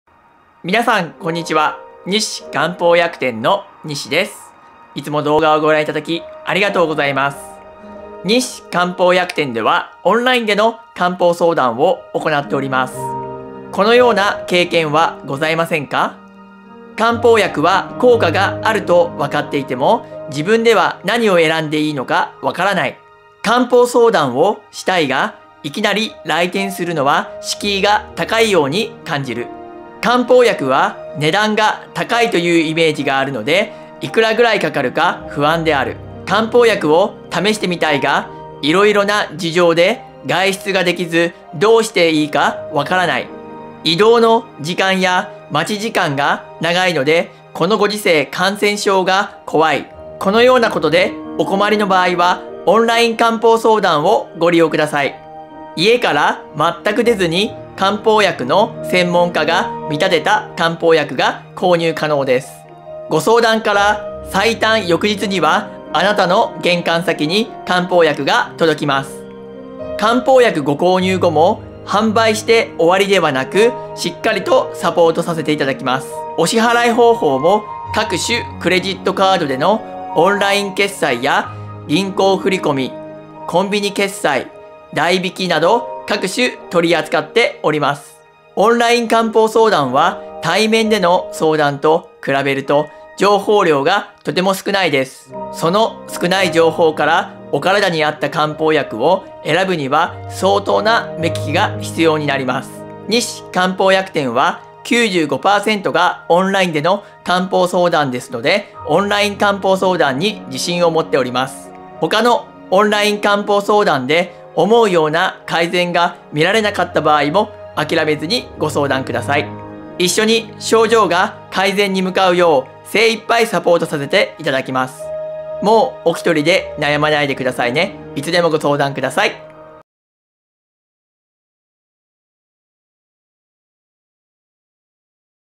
【西漢方薬店】オンライン漢方相談のご紹介
臨床歴16年の経験を活かし、子供からご高齢の方々の幅広い世代のお悩み、病気の改善のお手伝いをさせていただきます。